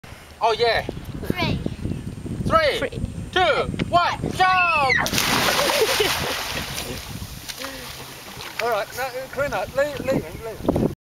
14-23 Jan 2011 - Michael Jumps into the sea
I don't know how many times that Michael jumped in the sea... crazy ! but he had a great fun!